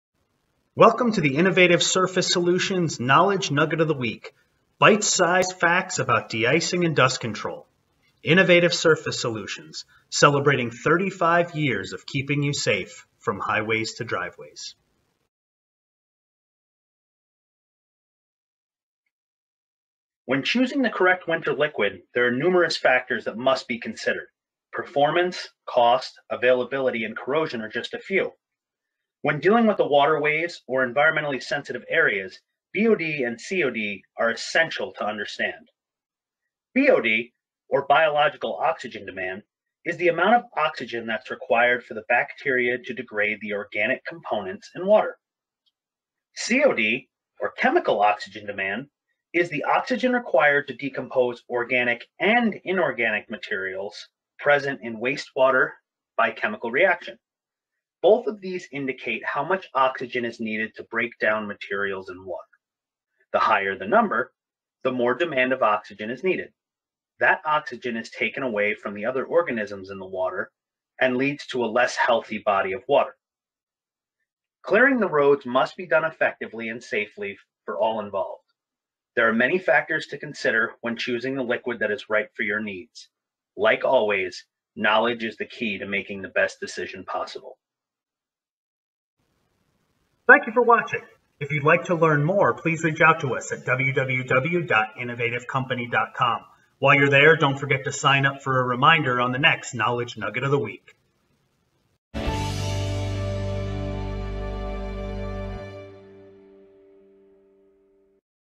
Understanding BOD & COD
Learn what BOD and COD testing is and why those numbers need to be considered when choosing your winter liquids.